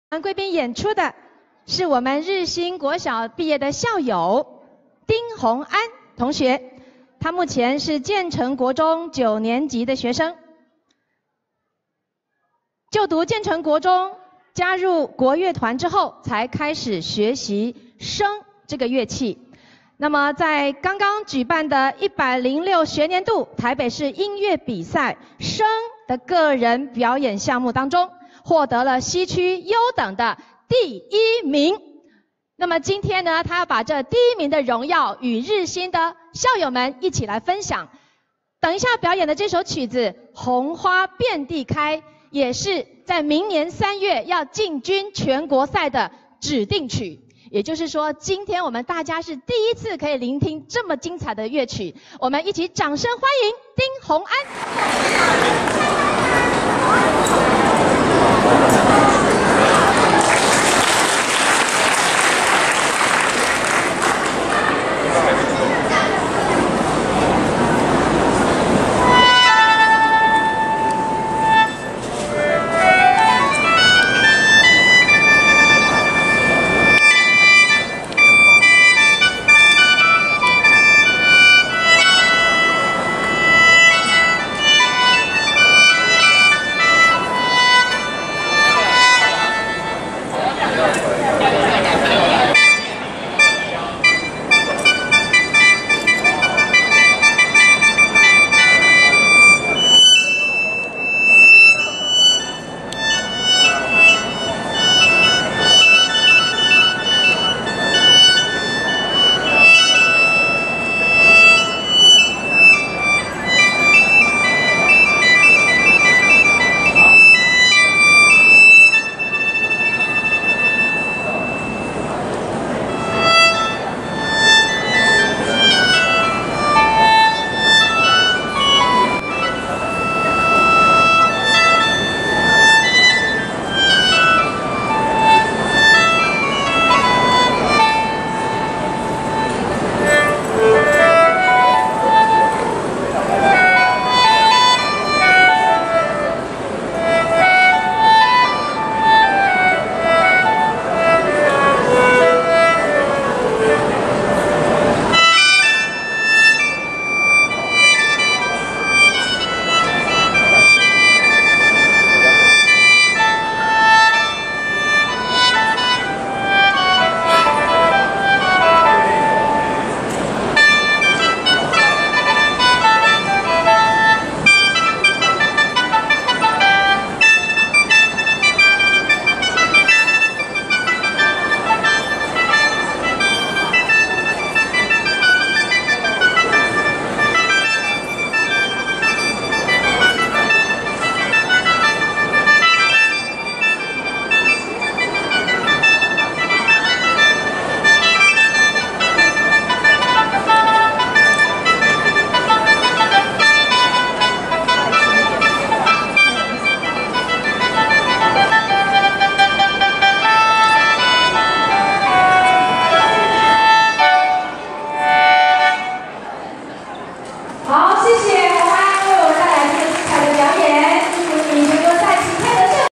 日新百年校慶 笙獨奏
2017/11/11 日新國小創校百週年慶祝大會 丁小寶笙獨奏